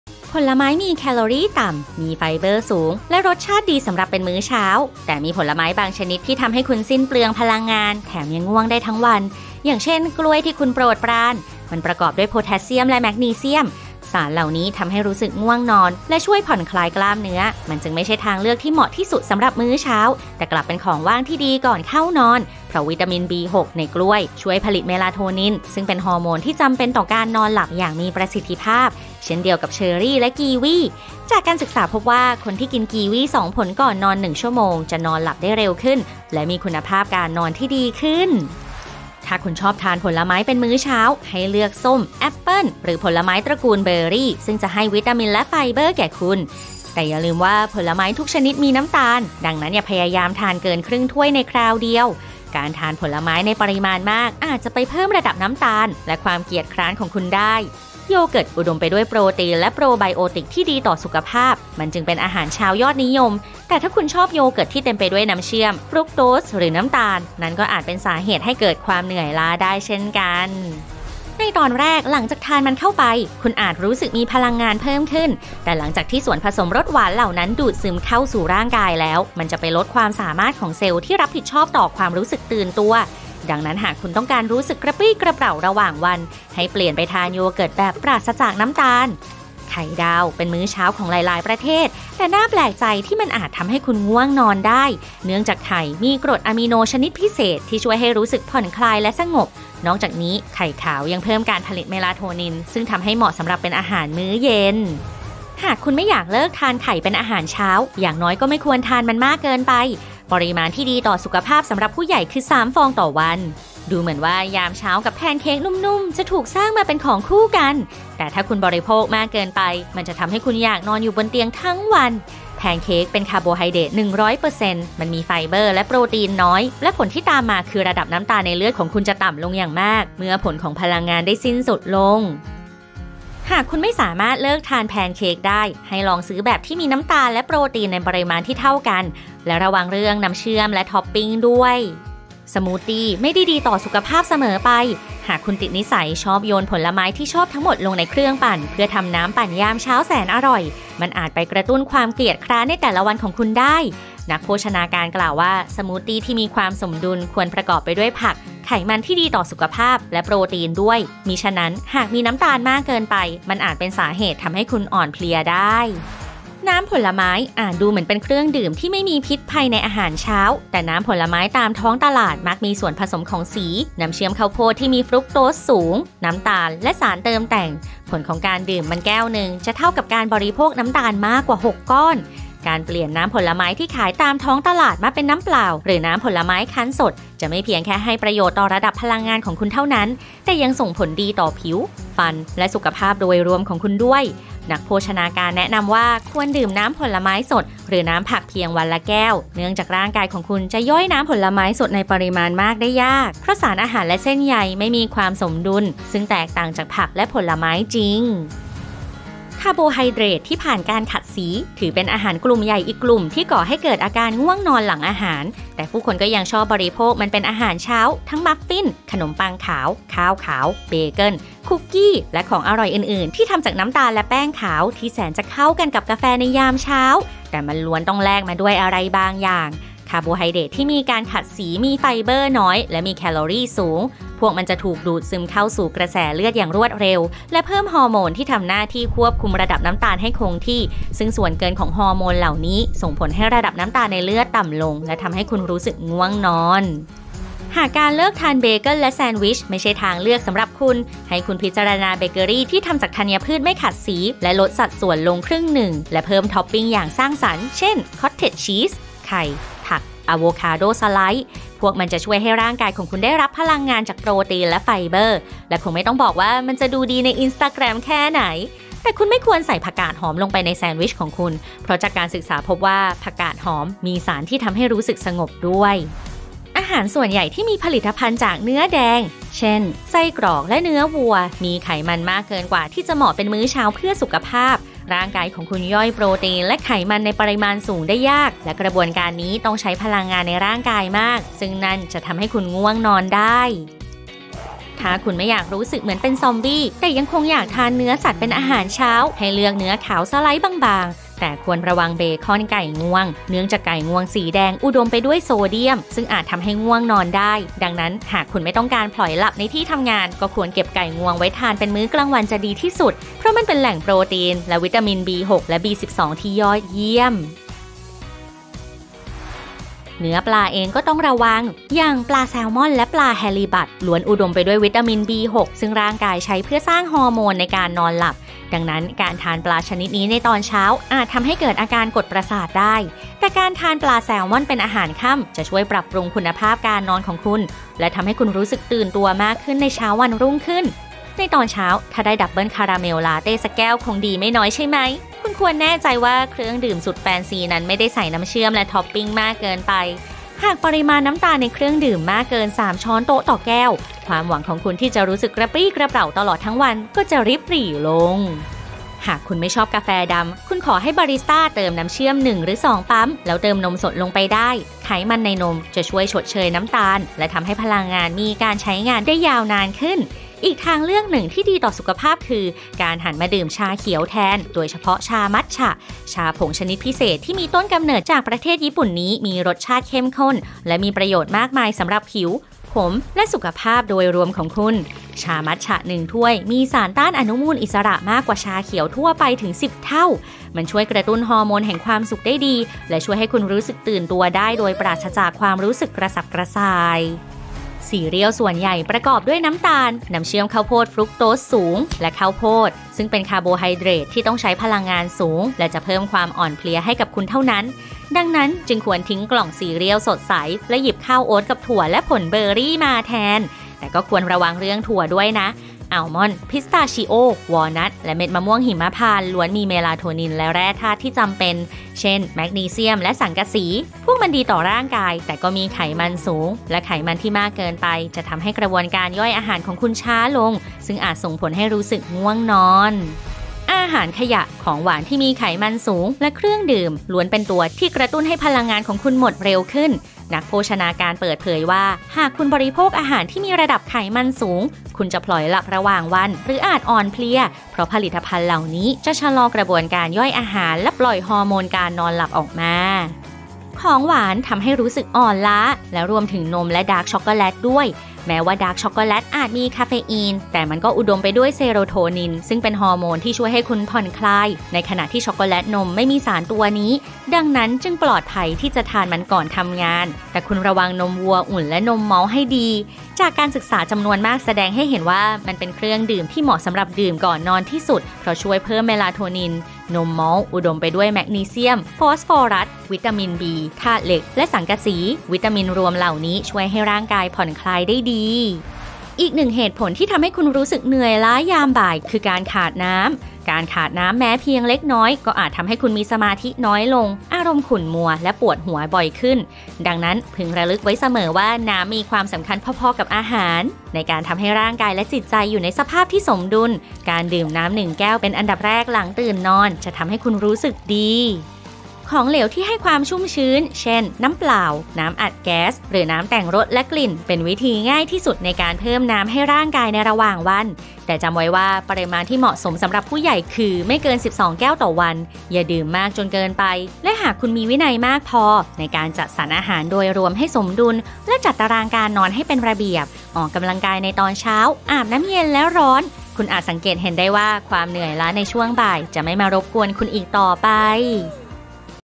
เพราะแบบนี้ไง คุณถึงรู้สึกง่วงหลังทานอาหาร
ว่ากันว่าอาหารเช้าเป็นมื้อที่สำคัญที่สุดของวันซึ่งนั่นไม่ใช่แค่คำกล่าวลอย ๆ แต่อาหารที่คุณเลือกรับประทานเป็นมื้อแรกของวันอาจทำให้พลังงานของคุณหมดได้! ตัวอย่างเช่น อาหารหลายชนิดที่มีน้ำตาล เช่น ซีเรียลที่คุณชอบอาจกำลังทำให้พลังงานของคุณถดถอยลง ดังนั้นได้เวลาทบทวนเมนูอาหารเช้าของคุณแล้ว... เราได้รวบรวมอาหารทั่วไป 16 ชนิดที่จะทำให้คุณอ่อนล้าทั้งวันมาบอกคุณตรงนี้แล้ว

อนิเมชั่นสร้างสรรค์โดย ชีวิตสดใส